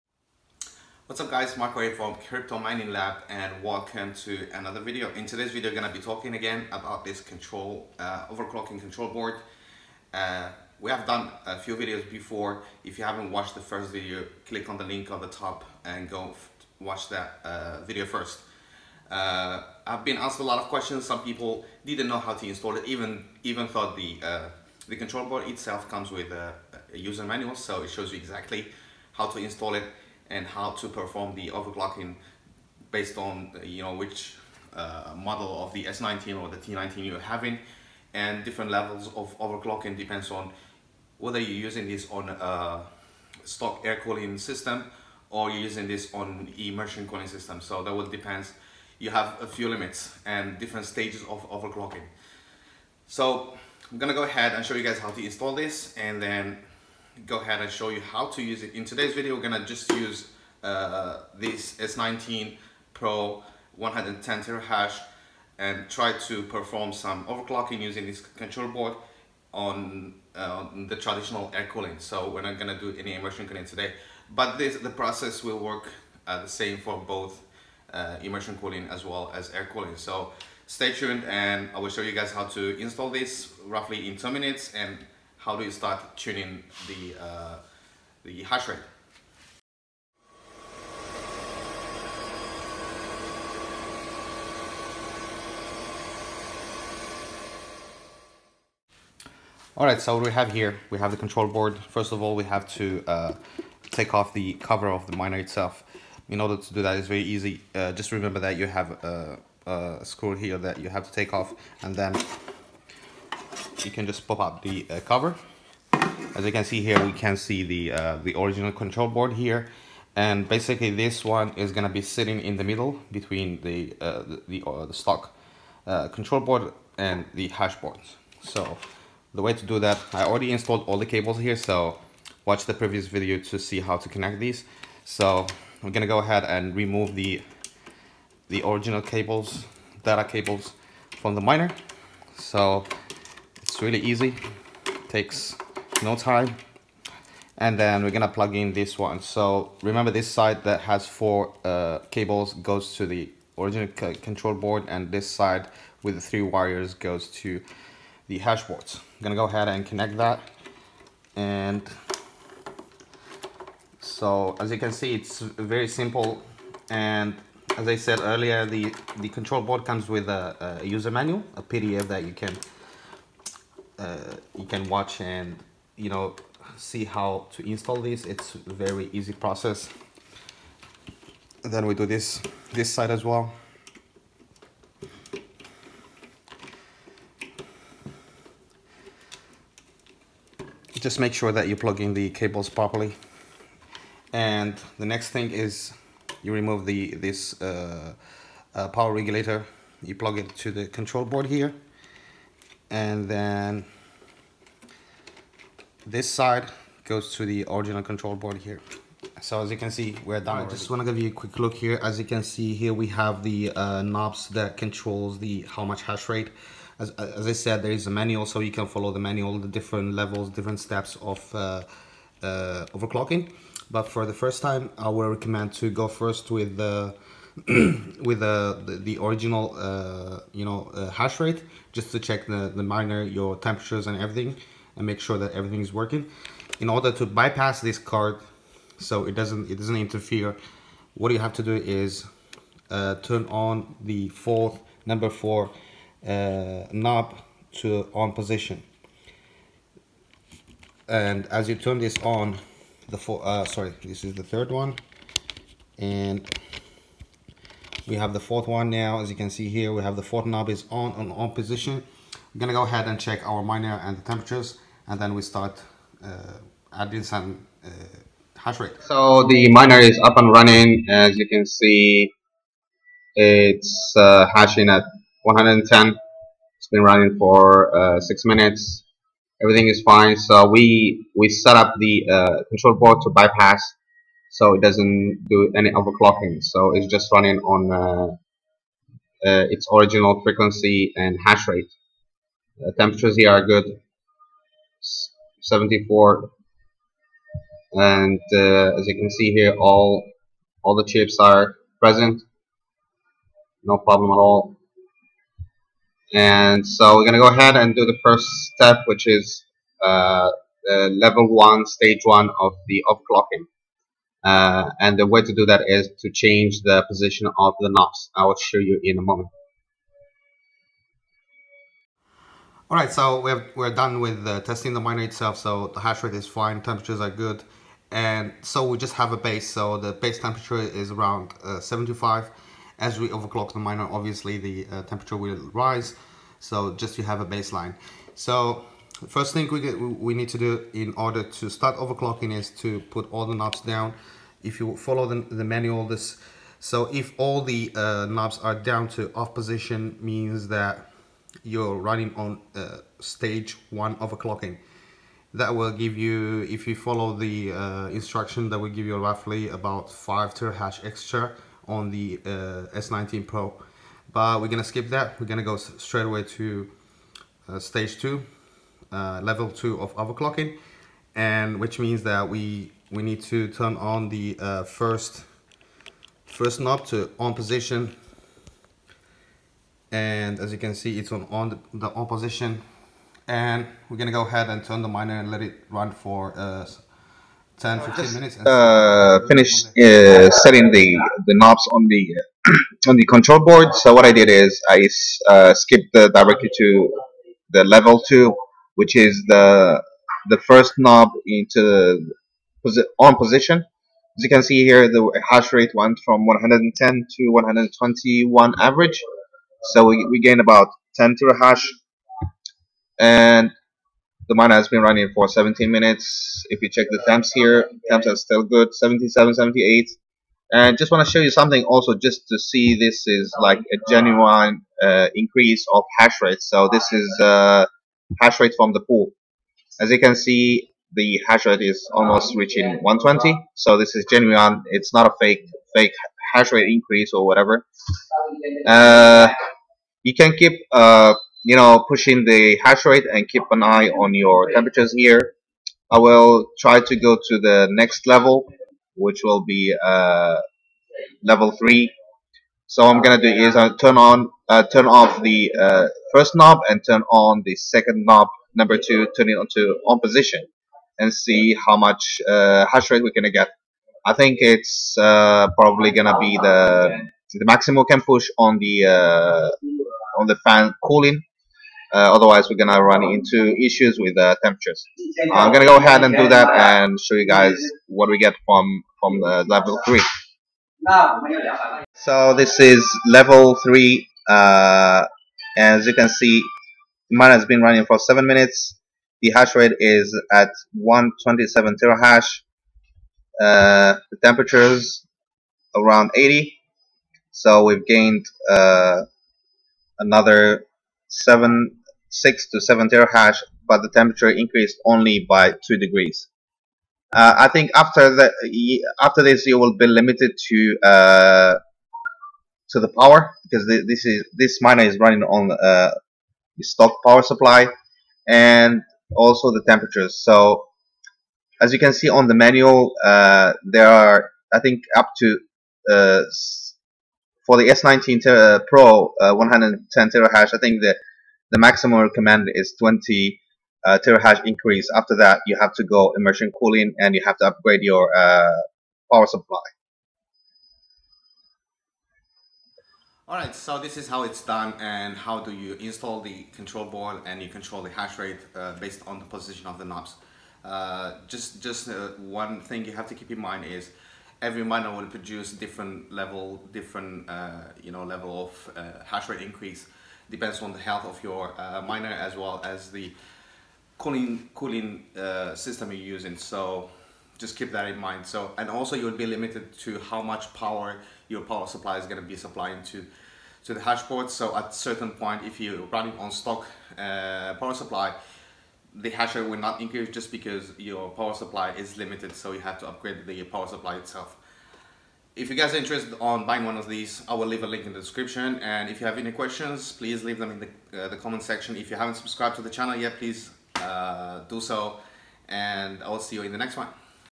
up to 40% gain with zero fees
Step by Step guide on how to increase the Hashrate on your S19 Pro with bitmain firmware
1- Hardware installation
2- testing before overclocking
3- different levels on overclocking capabilities